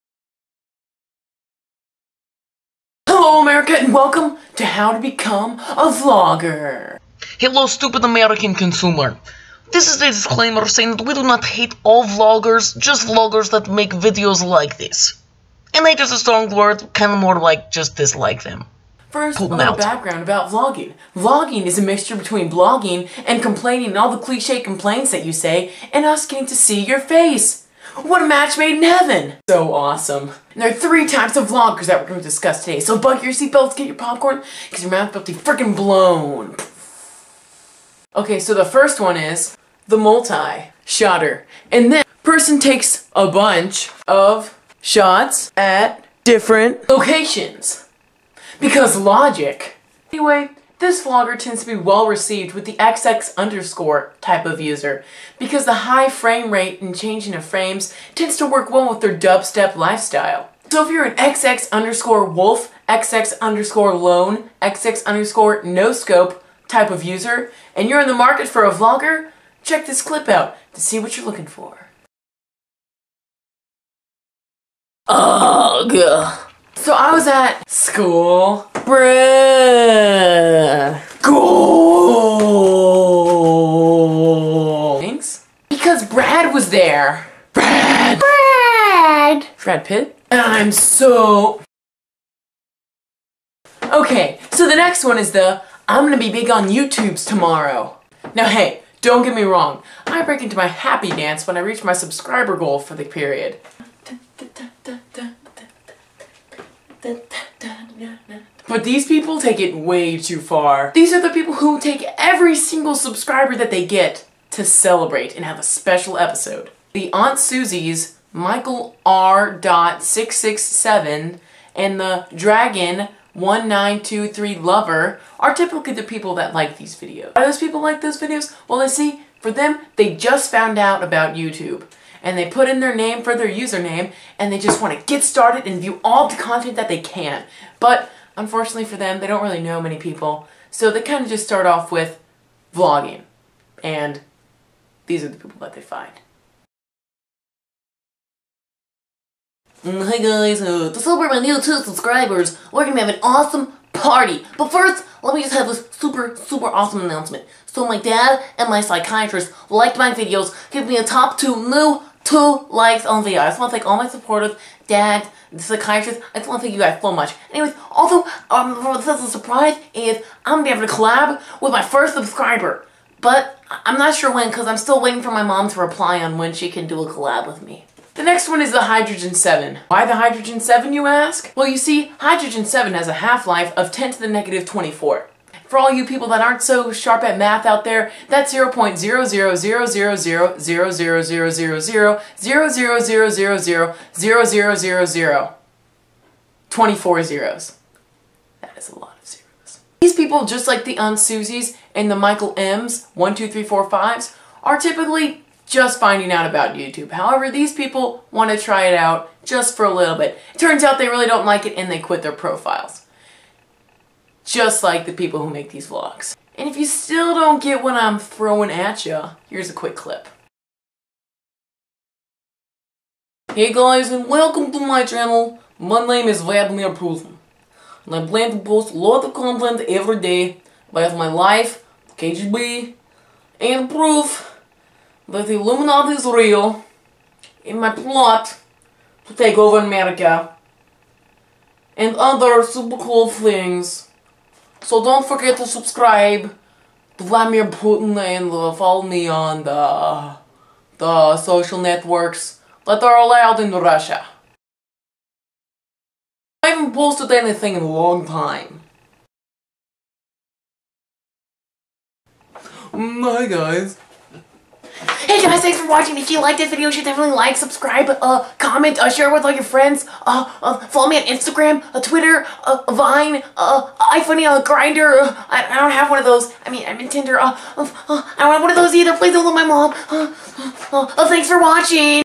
How To Be A Bad Vlogger
Have you ever wanted to be that guy who is going to be famous on the YouTubes? What about the fellow that has as much motivation as a dead sloth? Or maybe even the guy who has the attention span of Hydrogen-7? If so this video will eloquently demonstrate the proper habits of a BAD vlogger!!

Also make sure you click the videos to the left of your screen that are also by me because... uh... I'll give you twenty bro bucks?